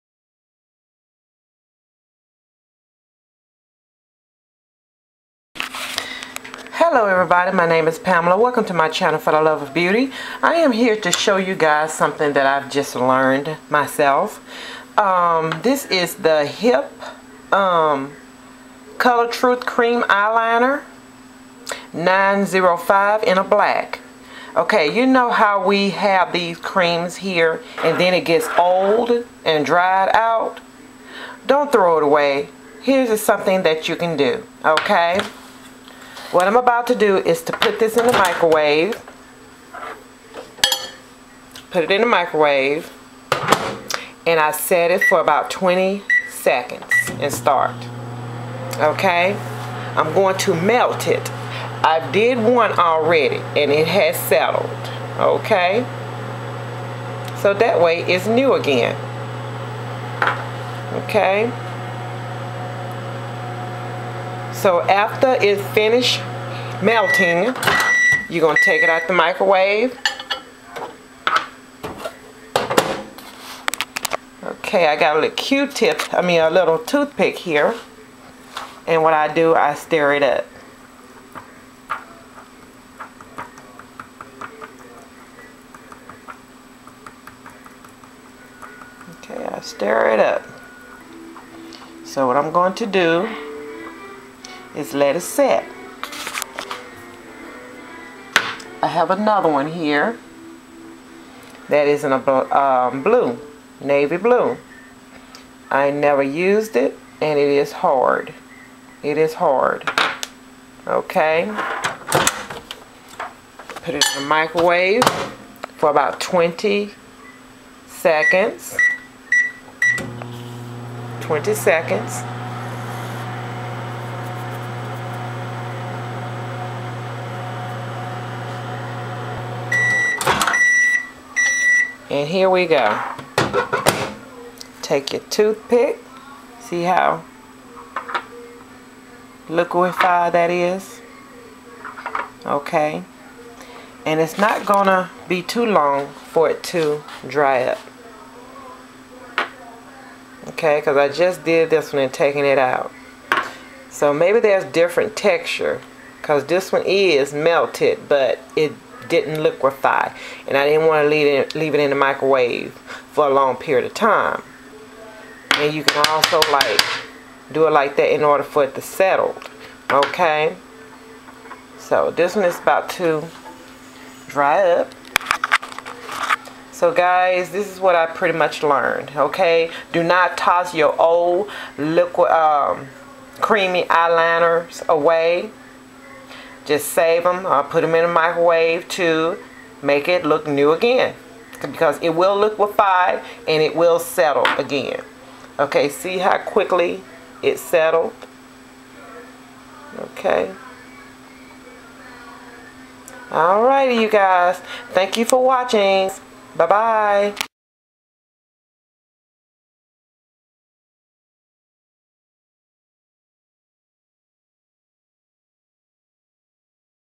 HOW TO REVIVE YOUR OLD CREAM EYELINERS
I was going to throw them away, but I thought about putting them in them in the microwave to make hen look new again and it worked. I sit in the microwave for about 20 secs until melted and then let the set. you can also add a little studio fix or a little visine to your eyeliner brush.